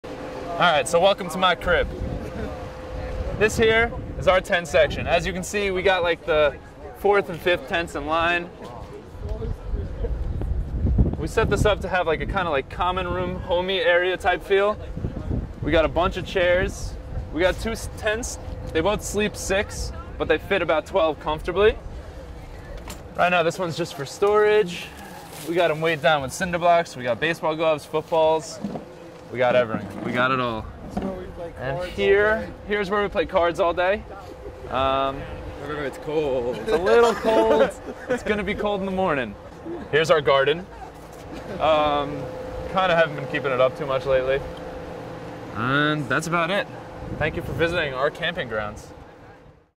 Tent Tour at Binghamton University
Take a tour of the tents set up outside the Binghamton Events Center as die-hard fans wait for seats to the men's America East Championship game.